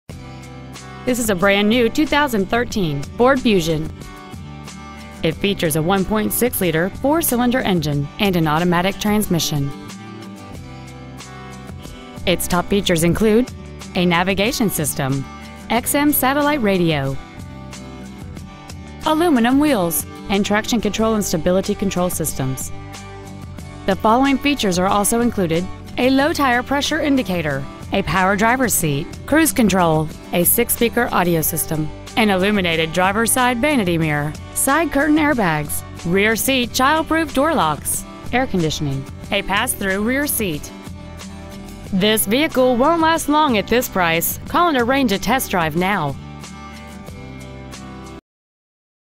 2013 FORD FUSION Highland Park IL
We have been honored to serve the Highland Park IL area , we promise that your experience at our dealership will exceed your expectations ! Year : 2013 Make : FORD Model : FUSION Engine : 1.6L I4 16V GDI DOHC TURBO Trans . : AUTOMATIC Exterior : STERLING GRAY Interior : CHARCOAL BLACK Stock : N10621 Highland Park Ford Lincoln Mercury Park Avenue West Highland Park , IL 60035